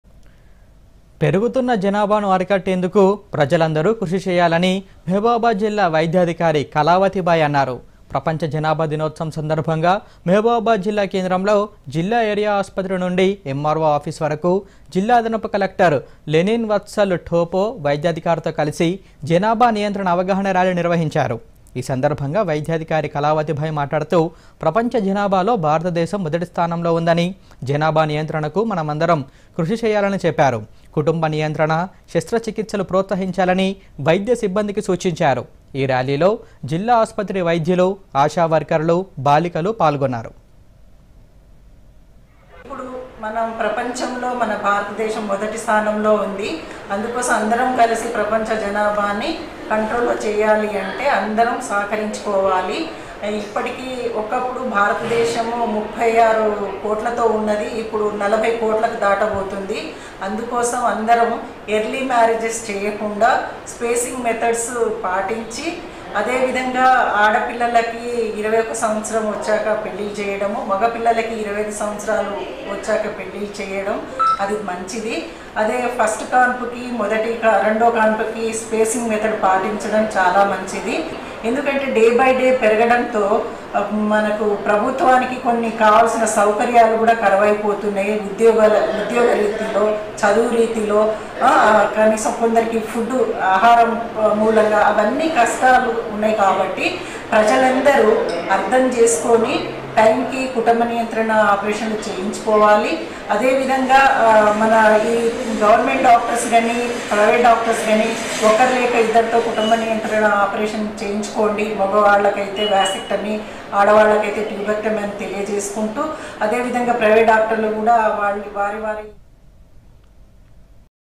మహబూబాబాద్ జిల్లా కేంద్రంలో నిర్వహించిన ప్రపంచ జనాభా దినోత్సవం | World Population Day | Mahabubabad
పెరుగుతున్న జనాభాను అరికట్టేందుకు ప్రజలందరూ కృషి చేయాలని మహబూబాబాద్‌ జిల్లా వైద్యాధికారి కళావతి బాయి అన్నారు. ప్రపంచ జనాభా దినోత్సవం సందర్భంగా మహబూబాబాద్ జిల్లా కేంద్రంలో జిల్లా ఏరియా ఆసుపత్రి నుండి ఎమ్మార్వో ఆఫీస్ వరకు జిల్లా  అదనపు కలెక్టర్ లెనిన్ వత్సల్ టోప్పో, వైద్యాధికారితో కలిసి జనాభా నియంత్రణ అవగాహన ర్యాలీ నిర్వహించారు.

ఈ సందర్భంగా వైద్యాధికారి కళావతి బాయి మాట్లాడుతూ. ప్రపంచ జనాభాలో భారతదేశం మొదటి స్థానంలో ఉందని, జనాభా నియంత్రణకు మనందరం కృషి చేయాలని చెప్పారు. కుటుంబ నియంత్రణ శస్త్ర చికిత్సలు ప్రోత్సహించాలని వైద్య సిబ్బందికి సూచించారు. ఈ ర్యాలీలో జిల్లా ఆసుపత్రి వైద్యులు, ఆశా వర్కర్లు, బాలికలు పాల్గొన్నారు.